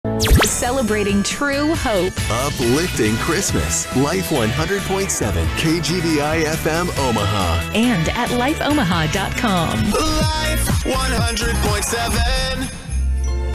KGBI - Life 100.7 - Station ID (8PM): December 8, 2023
The 8pm Station ID for KGBI-FM "Life 100.7" Omaha, Nebraska-Council Bluffs, Iowa. Copyright University of Northwestern – St. Paul.